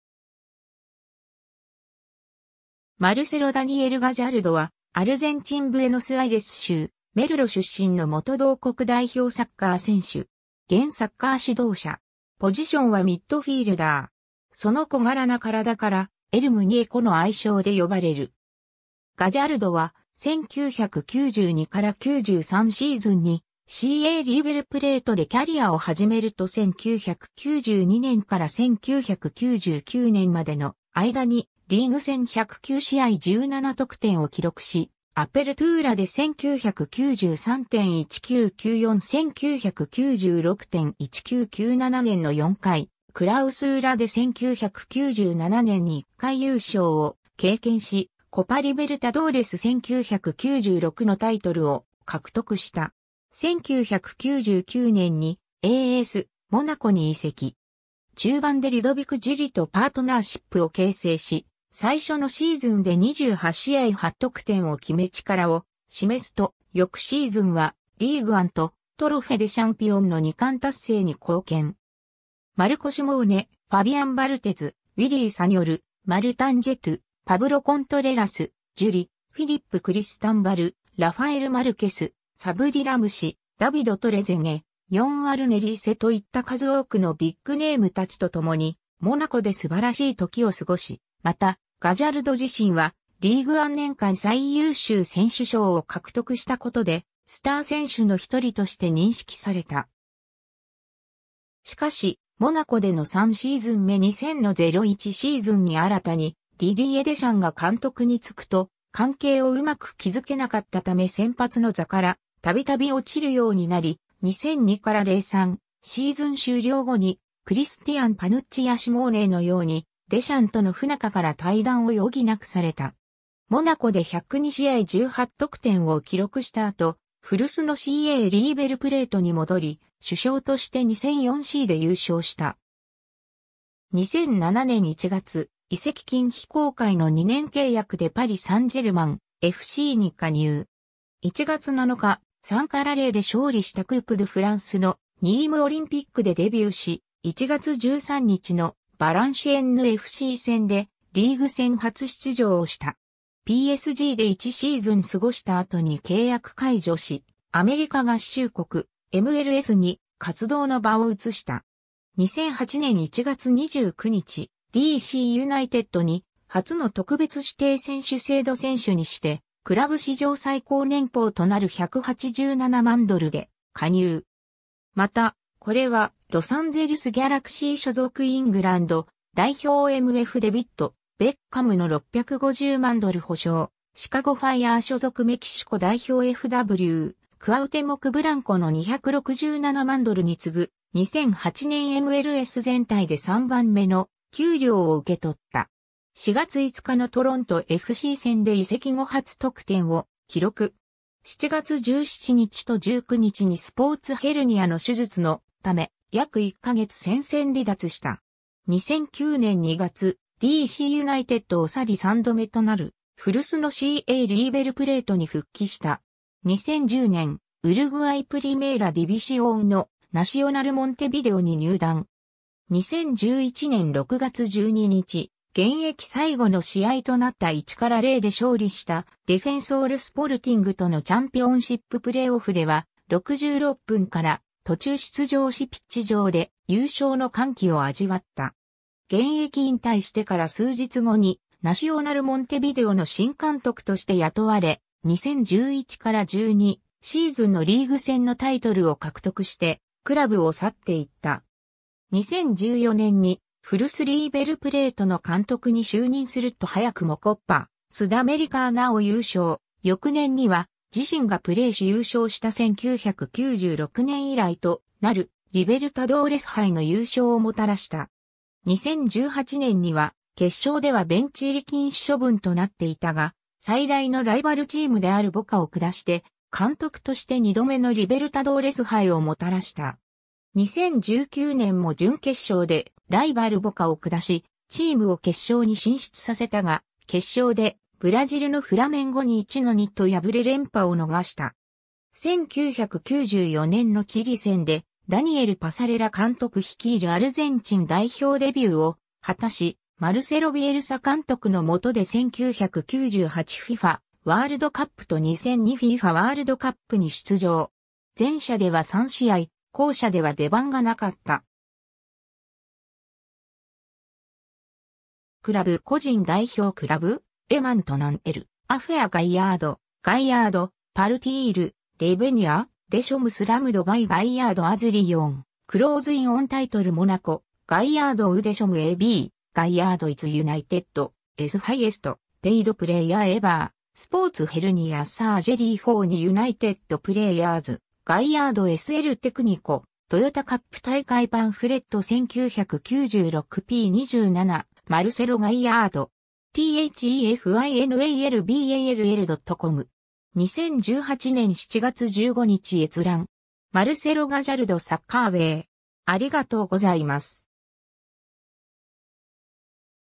マルセロ・ガジャルド
マルセロ・ダニエル・ガジャルド（Marcelo Daniel Gallardo スペイン語発音: [maɾˈselo ɣaˈʝaɾðo], 1976年1月18日 - ）は、アルゼンチン・ブエノスアイレス州メルロ出身の元同国代表サッカー選手。
現サッカー指導者。
その小柄な身体からエル・ムニェコ（El Muñeco、人形）の愛称で呼ばれる。
 ガジャルドは、1992-93シーズンにCAリーベル・プレートでキャリアを始めると1992年から1999年までの間にリーグ戦109試合17得点を記録し、アペルトゥーラで1993・1994・1996・1997年の4回、クラウスーラで1997年に1回優勝を経験し、コパ・リベルタドーレス1996のタイトルを獲得した。
 1999年にASモナコに移籍。
中盤でリュドヴィク・ジュリとパートナーシップを形成し、最初のシーズンで28試合8得点を決め力を示すと、翌シーズンはリーグ・アンとトロフェ・デ・シャンピオンの2冠達成に貢献。
マルコ・シモーネ、ファビアン・バルテズ、ウィリー・サニョル、マルタン・ジェトゥ、パブロ・コントレラス、ジュリ、フィリップ・クリスタンヴァル、ラファエル・マルケス、サブリ・ラムシ、ダヴィド・トレゼゲ、ヨン・アルネ・リーセといった数多くのビッグネーム達と共にモナコで素晴らしい時を過ごし、また、ガジャルド自身はリーグ・アン年間最優秀選手賞を獲得したことでスター選手の1人として認識された。
 しかし、モナコでの3シーズン目2000-01シーズンに新たにディディエ・デシャンが監督に就くと、関係を上手く築けなかったため先発の座から度々落ちるようになり、2002-03シーズン終了後にクリスティアン・パヌッチやシモーネのようにデシャンとの不仲 から退団を余儀なくされた。
 モナコで102試合18得点を記録した後、古巣のCAリーベル・プレートに戻り主将として2004Cで優勝した。
 2007年1月、移籍金非公開の2年契約でパリ・サンジェルマンFCに加入。
1月7日、3-0で勝利したクープ・ドゥ・フランスのニーム・オリンピックでデビューし、1月13日のヴァランシエンヌFC戦でリーグ戦初出場をした。
PSGで1シーズン過ごした後に契約解除し、アメリカ合衆国・MLSに活動の場を移した。
 2008年1月29日、D.C. ユナイテッドに初の特別指定選手制度選手にして、クラブ史上最高年俸となる187万ドル で加入。
また、これはロサンゼルス・ギャラクシー所属イングランド代表MFデビッド・ベッカムの650万ドル保証、シカゴ・ファイアー所属メキシコ代表FWクアウテモク・ブランコの267万ドルに次ぐ、2008年MLS全体で3番目の給料を受け取った。
4月5日のトロントFC戦で移籍後初得点を記録。
7月17日と19日にスポーツヘルニアの手術のため約1ヶ月戦線離脱した。
2009年2月、DCユナイテッドを去り3度目となる古巣のCAリーベル・プレートに復帰した。
 2010年、ウルグアイ・プリメーラ・ディビシオンのナシオナル・モンテビデオに入団。
2011年6月12日、現役最後の試合となった1-0で勝利したデフェンソール・スポルティングとのチャンピオンシップ・プレーオフでは、66分から途中出場しピッチ上で優勝の歓喜を味わった。
 現役引退してから数日後にナシオナル・モンテビデオの新監督として雇われ、2011-12シーズンのリーグ戦のタイトルを獲得してクラブを去っていった。
 2014年に古巣リーベルプレートの監督に就任すると早くもコパ・スダメリカーナを優勝、翌年には自身がプレーし優勝した1996年以来となるリベルタドーレス杯の優勝をもたらした。
2018年には決勝ではベンチ入り禁止処分となっていたが、最大のライバルチームであるボカを下して監督として2度目のリベルタドーレス杯をもたらした。
 2019年も準決勝でライバルボカを下し、チームを決勝に進出させたが、決勝でブラジルのフラメンゴに1-2と敗れ連覇を逃した。
 1994年のチリ戦でダニエル・パサレラ監督率いるアルゼンチン...